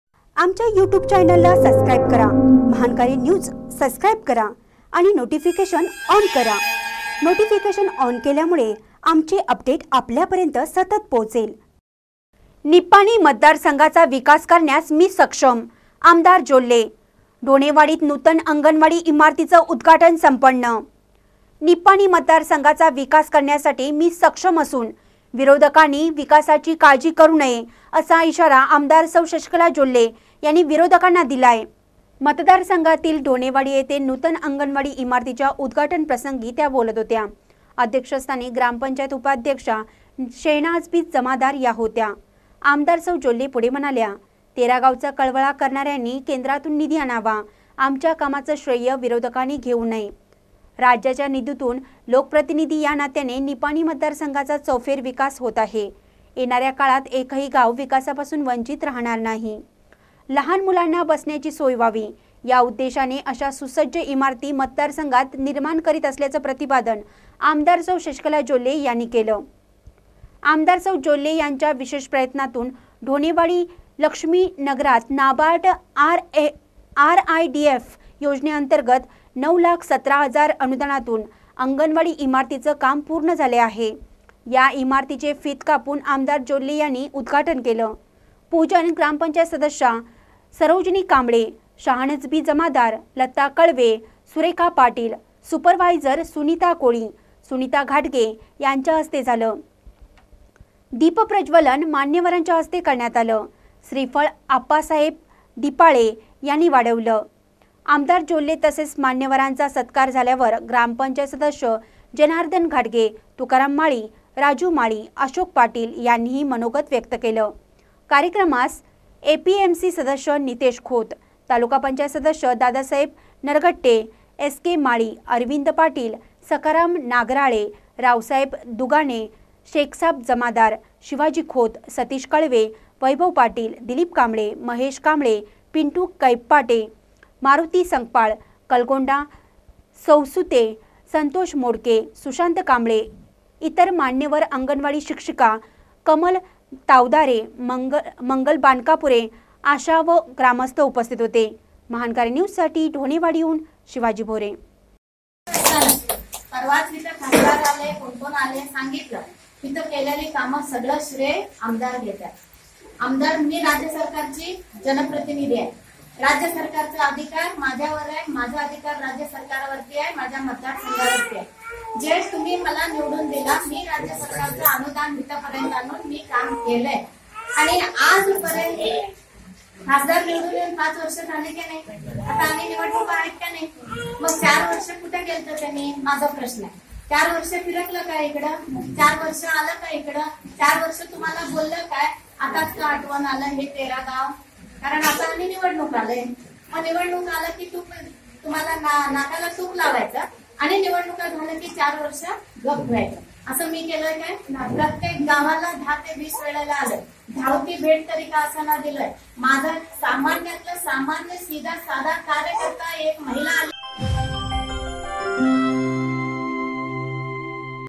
निपाणी मतदार संघाचा विकास करण्यास मी सक्षम- आ.जोल्ले
निपाणी मतदार संघाचा विकास करण्यास मी सक्षम- आ.जोल्ले : ढोणेवाडीत  नूतन अंगणवाडी इमारतीचे उदघाटन संपन्न